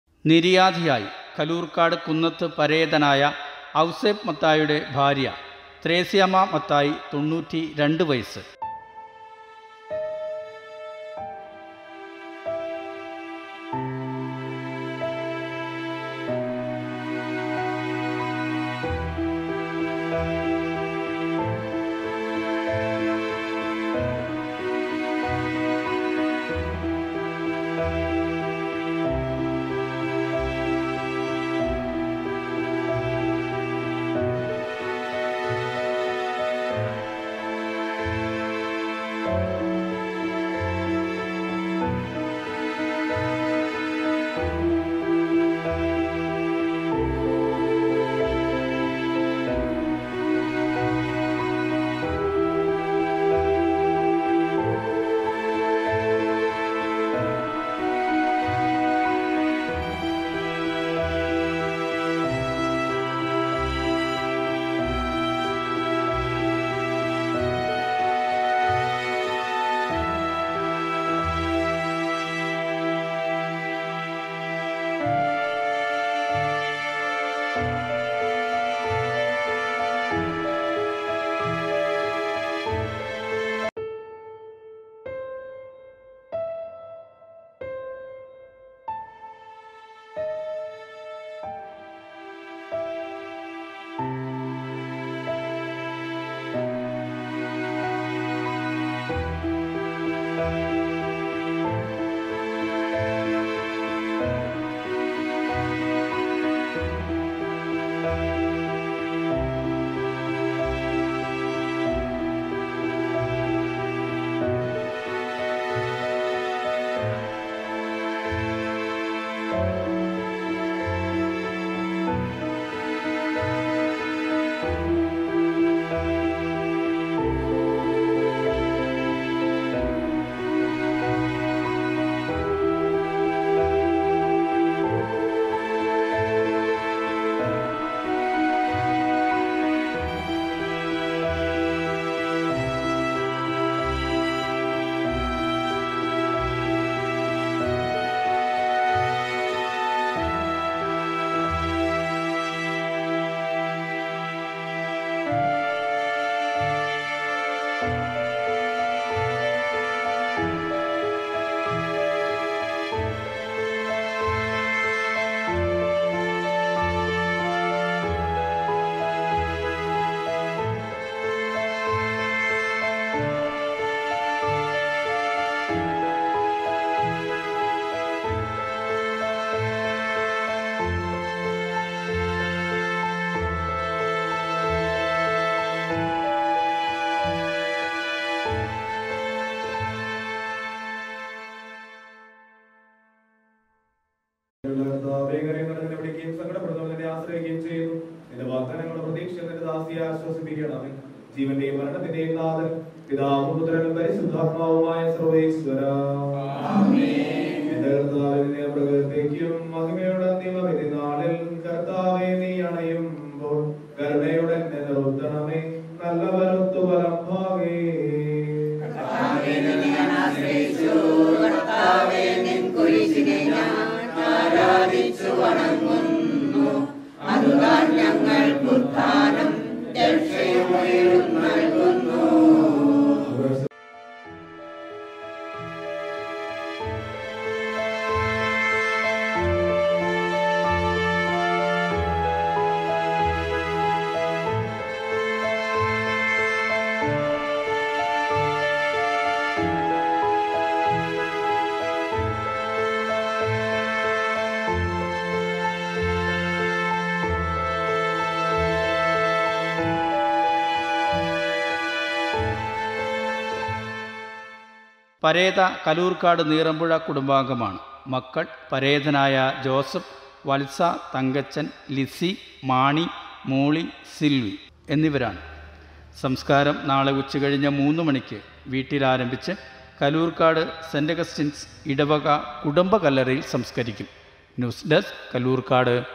കല്ലൂർക്കാട് .കുന്നത്ത് പരേതനായ ഔസേപ്പ്മത്തായിയുടെ ഭാര്യ ത്രേസ്യാമ്മ മത്തായി(92) നിര്യാതയായി
ശവസംസ്കാരം 05/07/2024 വെള്ളിയാഴ്ച
3 pm ന്‌ വീട്ടിൽ ആരംഭിച്ച് കല്ലൂർക്കാട് സെൻറ് അഗസ്റ്റിൻപള്ളിയിലെ കുടുംബ കല്ലറയിൽ നടത്തപ്പെടുന്നു. പരേത കല്ലൂർക്കാട് നീറമ്പുഴ കുടുംബാംഗമാണ്.
മക്കൾ
പരേതനായ ജോസഫ്,ബേബി, വൽസ,തങ്കച്ചൻ,ലിസി,
മാണി,മോളി,സില്‍വി.

മരുമക്കൾ
എൽസി തെങ്ങുംകുടിയിൽ, തങ്കമ്മ ചിറക്കൽ അരിയിക്കൽ, കുര്യാക്കോസ് തണ്ണിക്കോട്ട്,
സിസിലി മുടക്കാലിൽ ,പരേതനായ ജോസ് പാലിശ്ശേരി, റെനി കൊച്ചുമുട്ടം, ബേബി നെല്ലിക്കുന്നേൽ, ജോസ് അറുകാലിൽ . Np:മൃതദേഹം വൈകിട്ട് 5 30 pm ന് വീട്ടിൽ കൊണ്ടുവരുംTDCKERALA kalloorkad # TDC #